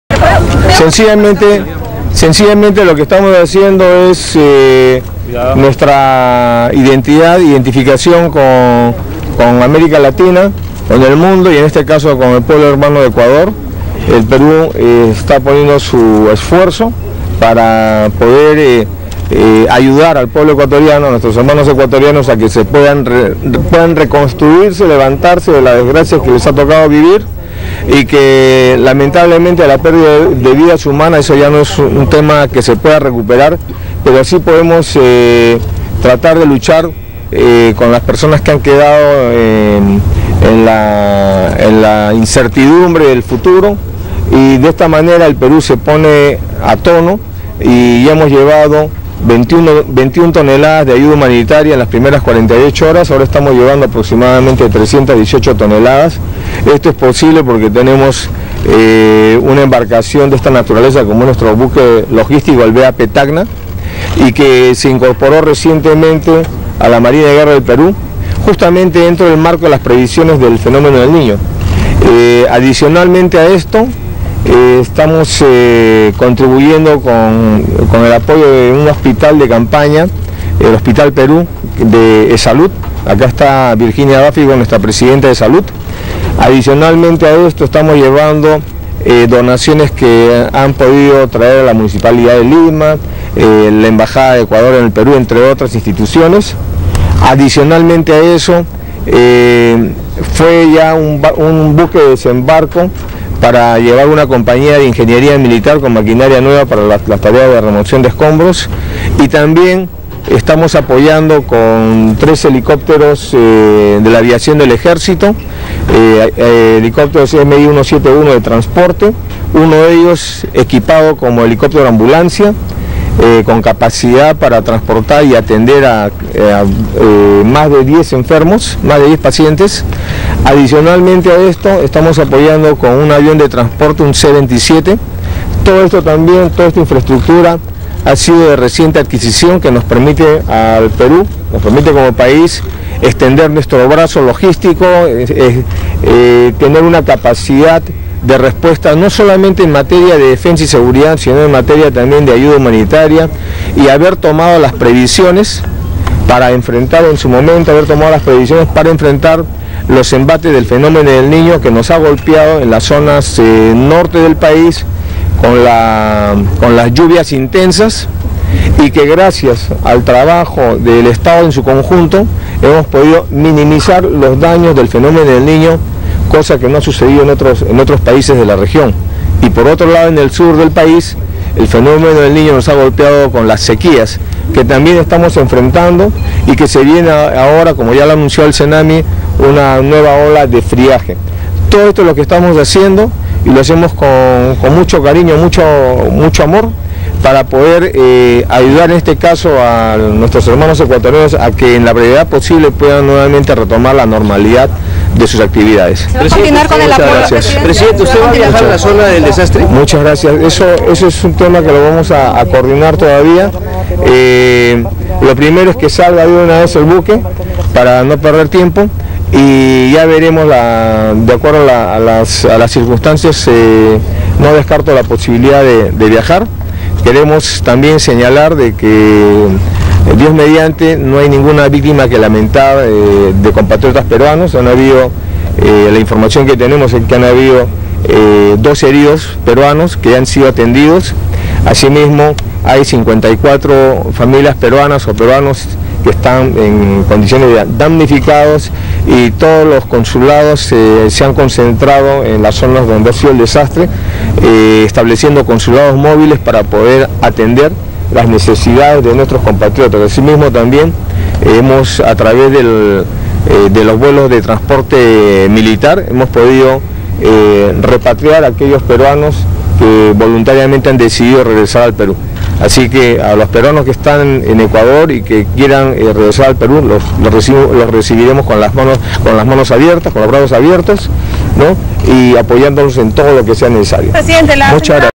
Zarpe del Buque Armada Peruana BAP “Tacna” con destino a Ecuador
Presidente Ollanta Humala encabezó el zarpe del Buque Armada Peruana BAP “Tacna” con destino a Ecuador, a donde llevará ayuda logística para los pueblos afectados por el terremoto del pasado sábado 16 de abril.